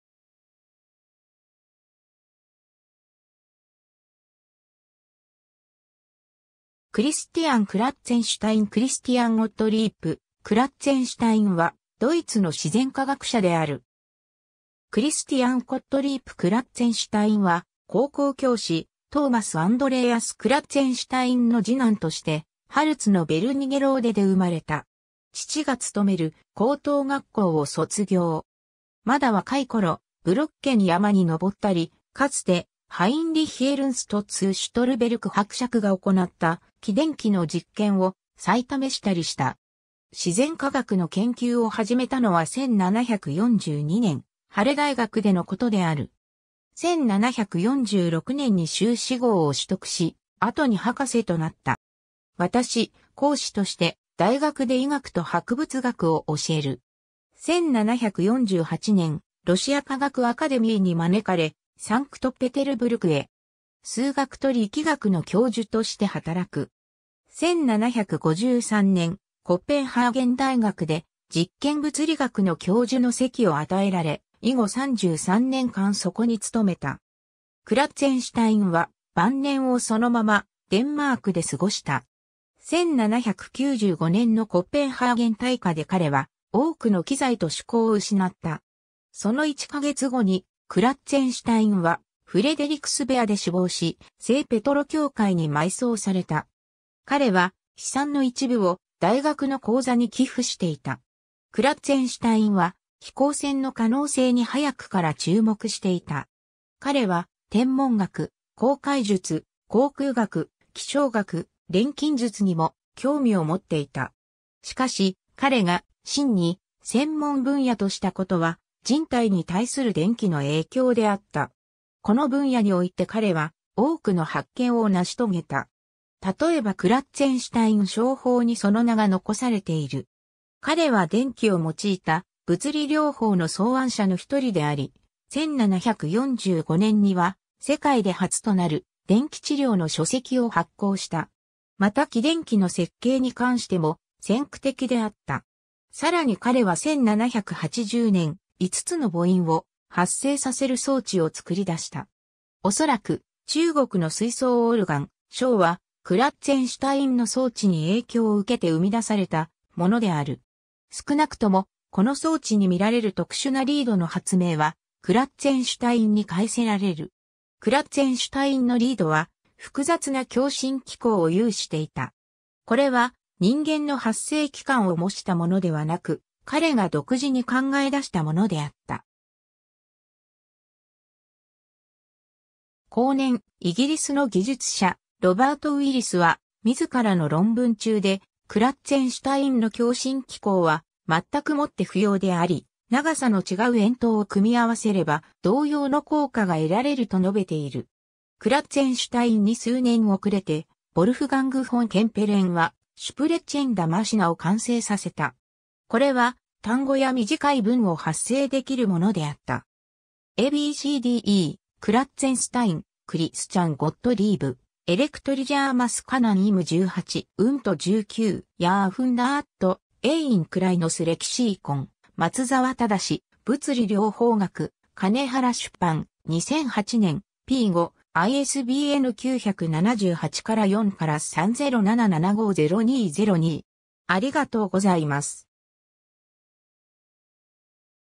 クリスティアン・クラッツェンシュタイン
マルティン・ルター大学ハレ・ヴィッテンベルクの教員
クリスティアン・クラッツェンシュタイン クリスティアン･ゴットリープ・クラッツェンシュタイン（Christian Gottlieb Kratzenstein、1723年1月30日 - 1795年7月6日）はドイツの自然科学者である。
 クリスティアン・コットリープ・クラッツェンシュタインは、高校教師トーマス・アンドレーアス・クラッツェンシュタインの次男としてハルツのヴェルニゲローデ（Wernigerode）で生まれた。
父が勤める高等学校を卒業。
まだ若い頃、ブロッケン山に登ったり、かつてハインリッヒ・エルンスト・ツー・シュトルベルク伯爵（Heinrich Ernst zu Stolberg）が行なった起電機の実験を再試したりした。
自然科学の研究を始めたのは1742年、ハレ大学でのことである。
1746年に修士号を取得し、後に博士となった。
私講師として大学で医学と博物学を教える。
1748年、ロシア科学アカデミーに招かれサンクトペテルブルクへ。
数学と力学の教授として働く。
1753年、コペンハーゲン大学で実験物理学の教授の席を与えられ、以後33年間そこに勤めた。
クラッツェンシュタインは晩年をそのままデンマークで過ごした。
1795年のコペンハーゲン大火で彼は多くの器材と手稿を失った。
その一ヵ月後にクラッツェンシュタインはフレデリクスベア（Frederiksberg）で死亡し、聖ペトロ教会（St. Petrikirche）に埋葬された。
彼は資産の一部を大学の講座に寄付していた。
 クラッツェンシュタインは飛行船の可能性に早くから注目していた。
彼は天文学、航海術、航空学、気象学、錬金術にも興味を持っていた。
しかし彼が真に専門分野としたことは、人体に対する電気の影響であった。
この分野において彼は多くの発見を成し遂げた。
例えばクラッツェンシュタイン小胞（Kratzensteinschen Bläschen）にその名が残されている。
彼は電気を用いた物理療法の創案者の一人であり、1745年には世界で初となる電気治療の書籍を発行した。
また起電機の設計に関しても先駆的であった。
 さらに彼は1780年（ペテルスブルク時代）、五つの母音（A、E、I、O、U）を発生させる装置を作り出した。
おそらく、中国の吹奏オルガン「笙」はクラッツェンシュタインの装置に影響を受けて生み出されたものである。
少なくとも、この装置に見られる特殊なリードの発明はクラッツェンシュタインに帰せられる。
（→音声合成） クラッツェンシュタインのリードは複雑な共振機構を有していた。
これは人間の発声器官を模したものではなく、彼が独自に考え出したものであった。
後年（1832年）、イギリスの技術者ロバート・ウィリス（Robert Willis）は自らの論文中で、クラッツェンシュタインの共振機構は全くもって不要であり、長さの違う円筒を組み合わせれば同様の効果が得られると述べている。
 クラッツェンシュタインに数年遅れて、ヴォルフガング・フォン・ケンペレンは"Sprechende Maschine"（話す機械）を完成させた。
これは単語や短い文を発声できるものであった。
 ^ a b c d e "Kratzenstein, Christian Gottlieb",Elektrisiermaschinen im 18. und 19. Jahrhundert - Ein kleines Lexikon（2009年4月18日閲覧） ^ 松澤正『物理療法学』金原出版、2008年、p5、ISBN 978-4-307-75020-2